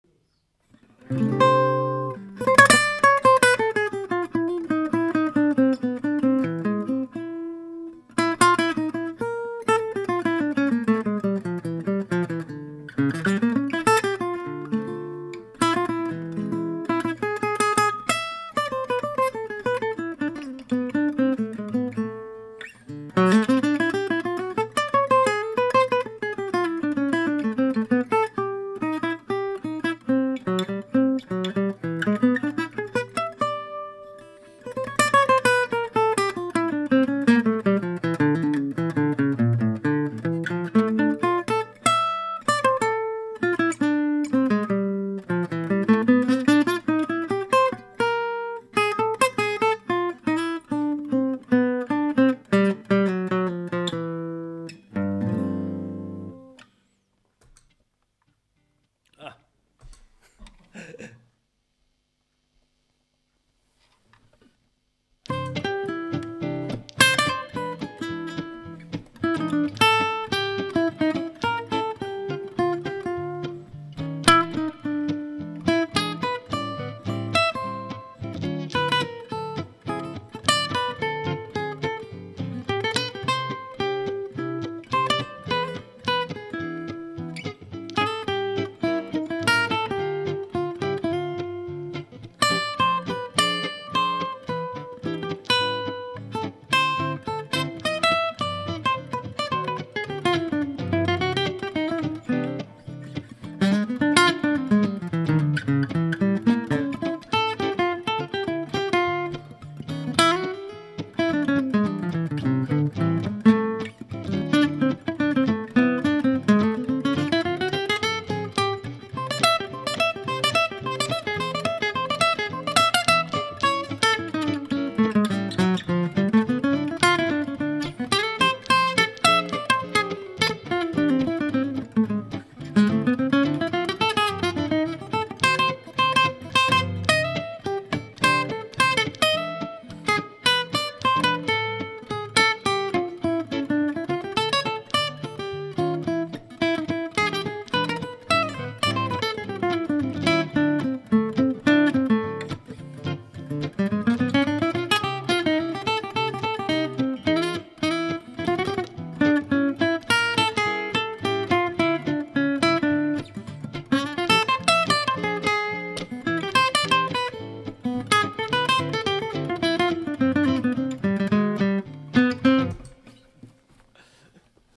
Roeder Gipsy style guitar from 1985 presented by Vintage Guitar Oldenburg & Martin Flindt
Vintage Guitar Oldenburg and Martin Flindt present a Roeder from 1985. The first 60 seconds recorded acoustically the rest played elctric and recorded from a Vibrolux 63 Reissue Amp with the Sennheiser E-906.